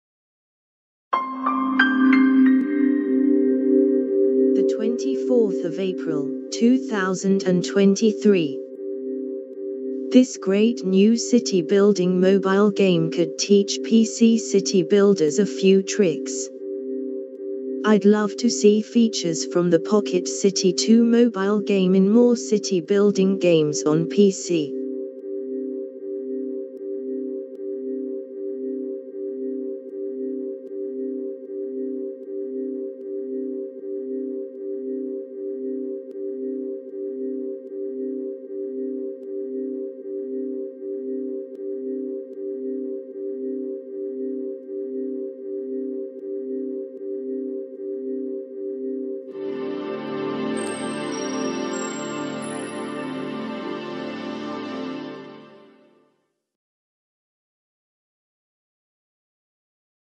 [Latest] This great new city building mobile game could teach PC city builders a few tricks
[Contents]
I'd love to see features from the Pocket City 2 mobile game in more city building games on PC.